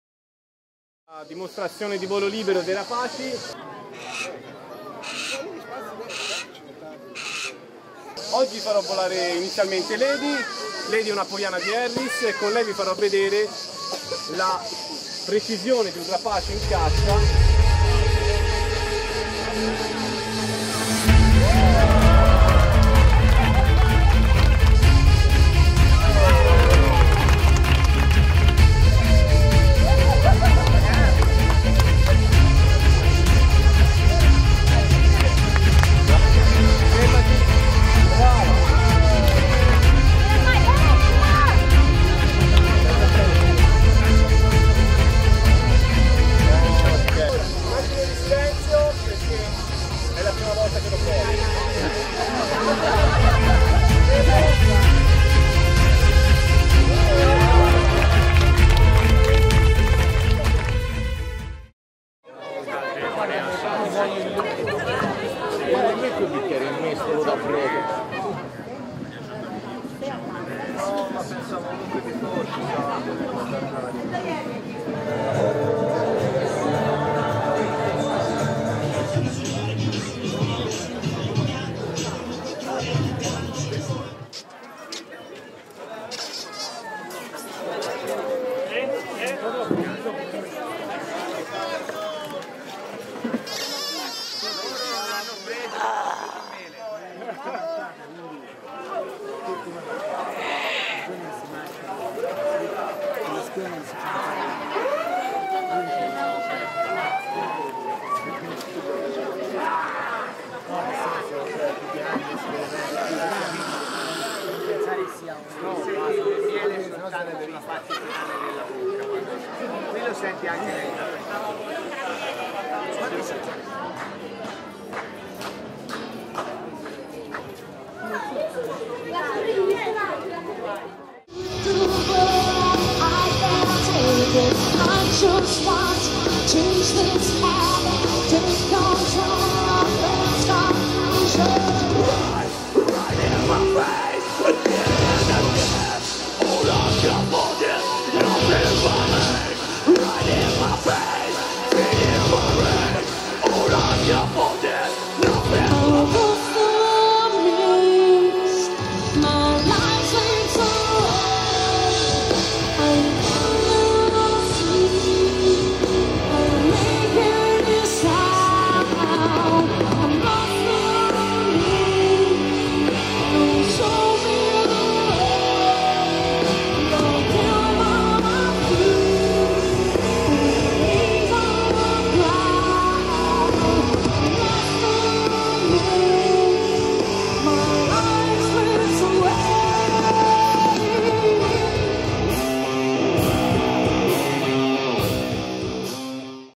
Festa dell' Unicorno 2016 - clips
Breve video della "Festa dell' Unicorno" edizione 2016 tenutasi a Vinci (FI) il 22-23-24 Luglio.
Vi si narra di falconeria e zombi.
Ho fatto pochissime riprese e questo è l'unico decente risultato ottenuto... XD